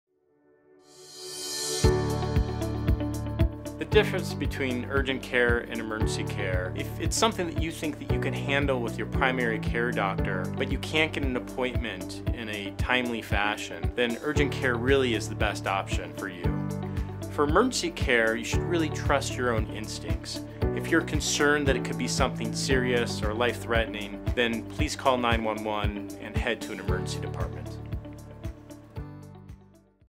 Urgent vs. Emergency Care (:30)
When it comes to getting care when you suddenly get sick or injured, it's important to know the difference between urgent vs. emergency care and what type of care you should seek out.

In this short video, Denver Health Downtown Urgent Care director Michael Breyer, M.D. explains when your illness or injury can be best treated at an urgent care and when you should call 911 and seek out emergency care.

Remember to always trust your instincts and if you are concerned it could be something serious or life-threatening, please call 911 immediately.

For more information on the Denver Health Downtown Urgent Care,